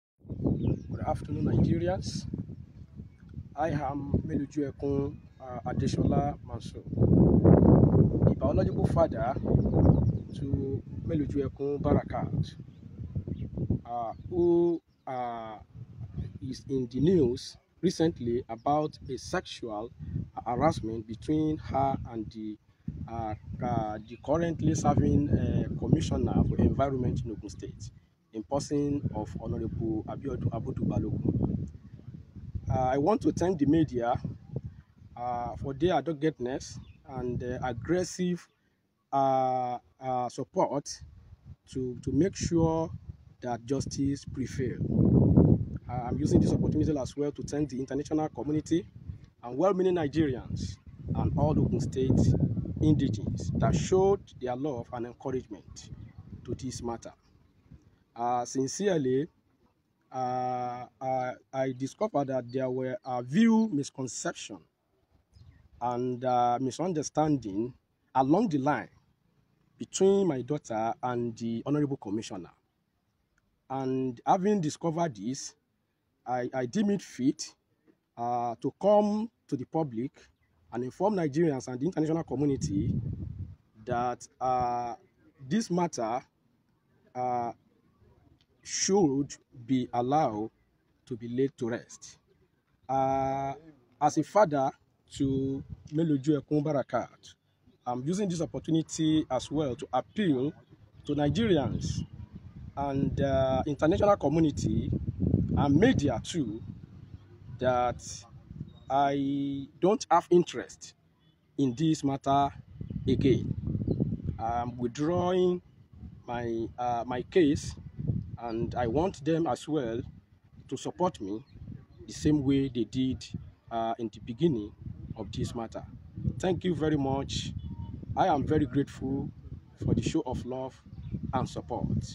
Barakat Mayowa Melojuekun Father Mr. Ade speaks on Ogun matter with Hon Comm
The father of the teenage girl Miss Barakat Mayowa Melojuekun speaks on the matter with Ogun State Comm for Enviroment teenager child adult misunderstanding @ogunstate